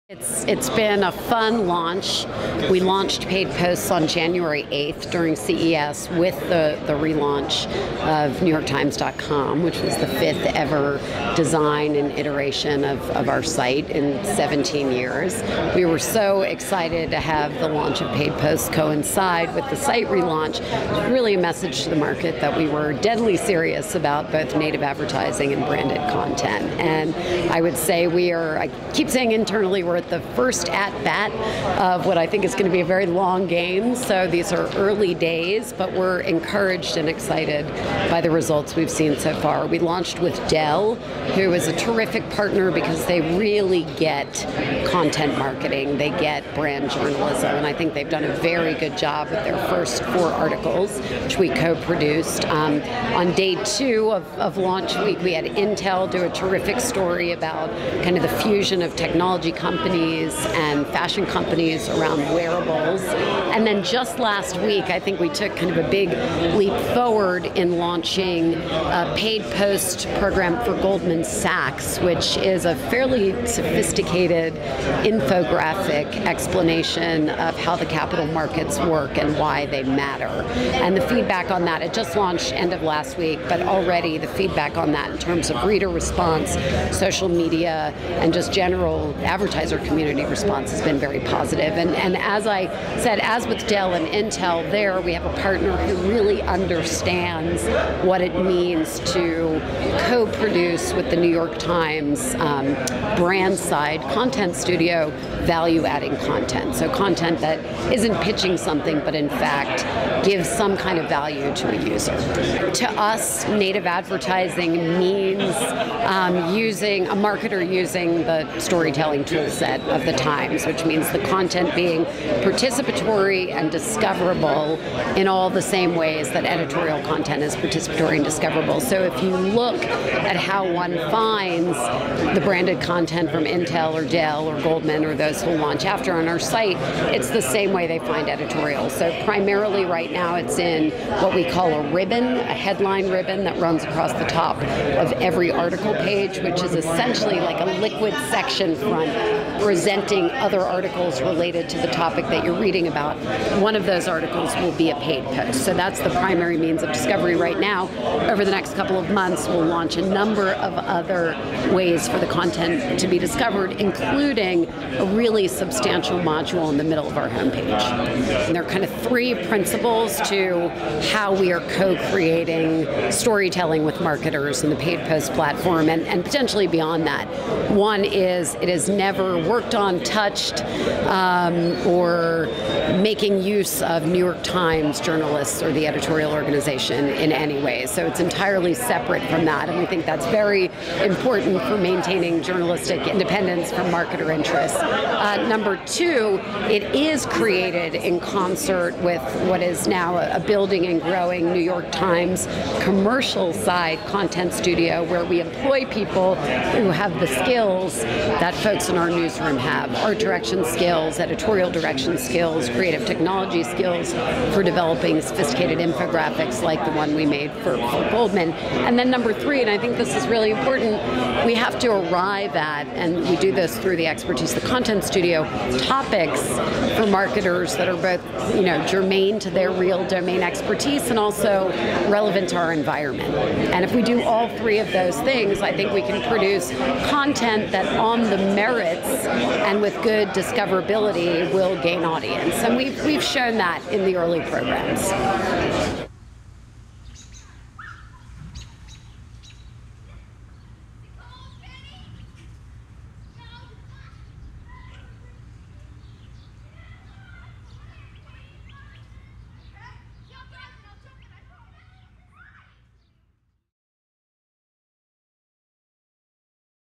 The New York Times Readies Front Page Module for Native Ads
PALM SPRINGS, CA - It is "early days" for paid posts, the new native advertising program just introduced by The New York Times, but it is set to grow and become increasingly discoverable, explains Meredith Levien, EVP for Advertising, in this interview with Beet.TV. The publisher has launched with campaigns for Dell, Intel and Goldman Sachs. Soon these sorts of campaigns will become more visible with the introduction of a large module in the middle of the front page, she says.